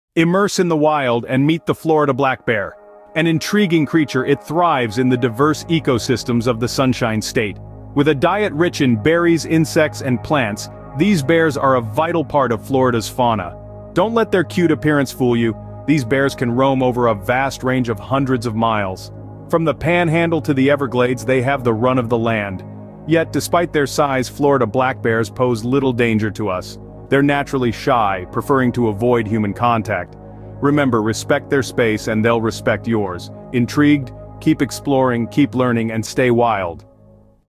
The Florida Black Bear - A Short Exploration
🐾 Embark on a fascinating journey into the wild heart of Florida with our latest video, "The Florida Black Bear - A Short Exploration." 🌲🐻 Discover the incredible world of the Florida black bear, a native species that calls the Sunshine State home.

🔍 In this short documentary, we delve into the unique habitat and behaviors of these magnificent creatures, shedding light on their conservation status and the crucial role they play in maintaining ecological balance. Learn about the challenges they face and the ongoing efforts to protect and preserve their natural environment.

📚 Whether you're a wildlife enthusiast, nature lover, or simply curious about Florida's diverse ecosystems, this video provides valuable insights into the lives of the iconic Florida black bears. Join us as we unravel the mysteries of their habitat, behavior, and conservation in this short yet captivating exploration.

👀 Don't miss out on this educational and visually stunning journey through the wilderness of Florida! 🌿🎥